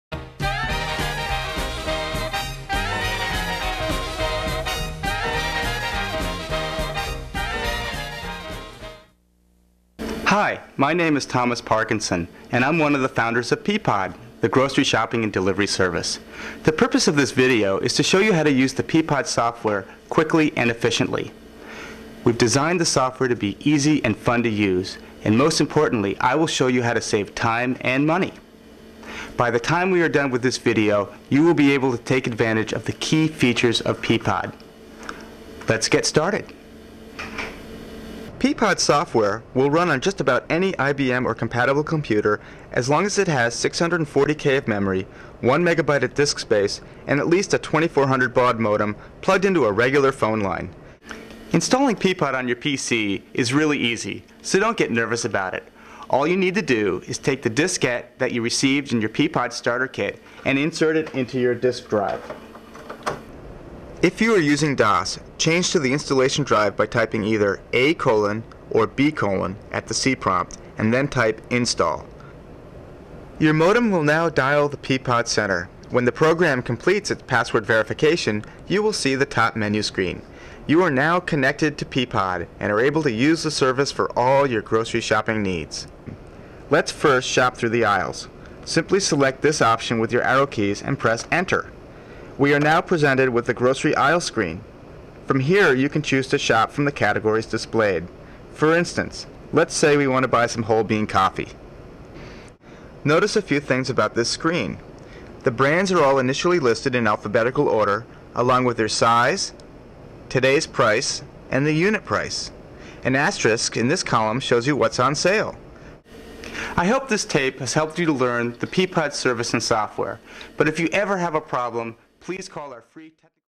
1993 Peapod PC Installation Very Short
Thomas Parkinson demonstrating installing the Peapod software in 1993.  This is the shortest version.